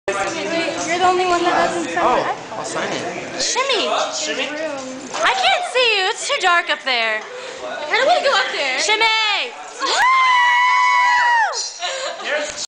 Tanner Patrick Shimmying
We stayed after the show, and he shimmied for us.
it was great. ;)
Sorry for my loud voice.
Check out the other Disco Curtis videos.